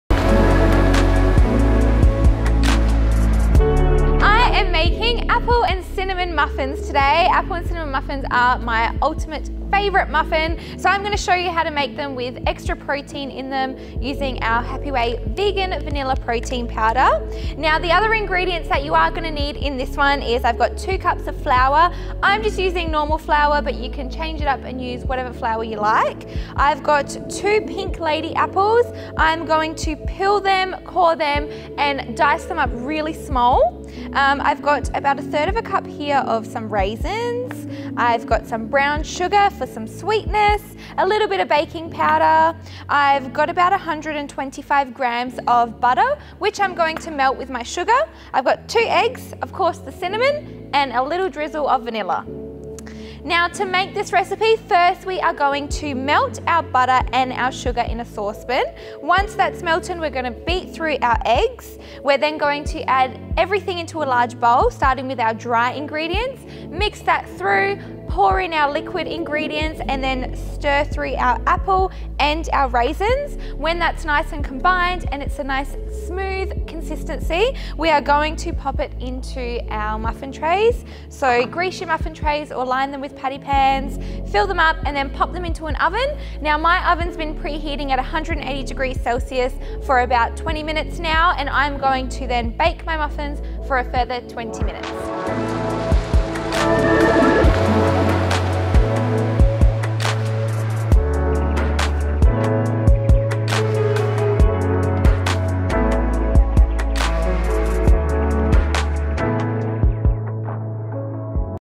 Apple & Cinnamon Muffins recipe | HAPPY WAY
Apple & Cinnamon Muffins

INGREDIENTS:
▫️2 apples, cored, peeled & cubed
▫️1/3 cup almond milk
▫️1 scoop Vegan Vanilla Happy Way Protein Powder

DEETS:

▫️Pre-heat oven to 180C fan-forced and line your muffin tray.

▫️In a saucepan over low heat melt the butter and sugar together until well combined. Beat the eggs and set aside.

▫️In a large bowl add all the dry ingredients and mix well. Pour in all the wet ingredients and mix well until a smooth batter forms.

▫️Spoon the batter into prepared muffin tray and bake for 20mins or until golden. Allow to cool completely. Enjoy!